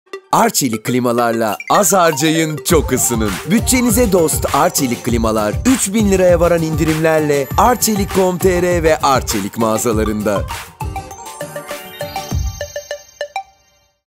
Arçelik klimalarla az harcayın çok ısının!
-15 derecede dahi ısıtan, enerji verimliliğiyle bütçenize dost Arçelik klimalarda 3.000 TL’ye varan indirim fırsatını kaçırmayın.

10 yıl kompresör garantili Arçelik klimalarla ilgili detaylı bilgi arcelik.com.tr ve Arçelik mağazalarında.